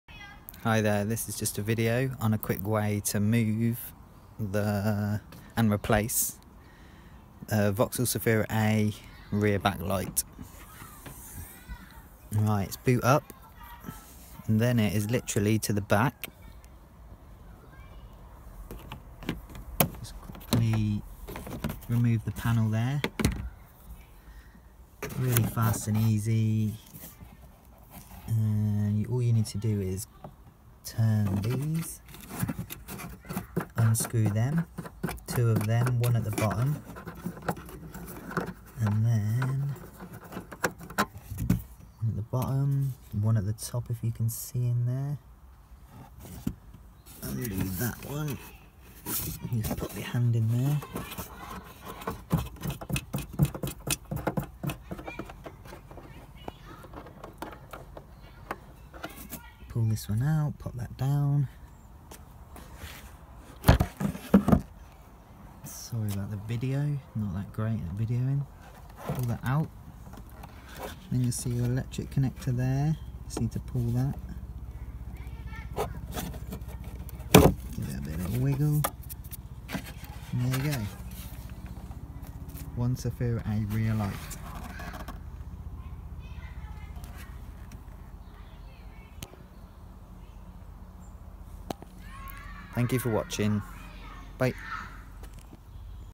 Vauxhall zafira A rear light removal 99-05
hi there , this is just a Quick video on how to remove rear light on a vauxhall zafira A , i hope this has helped in some way , if you could please subscribe to my channel and hit the like button that would be great , many thanks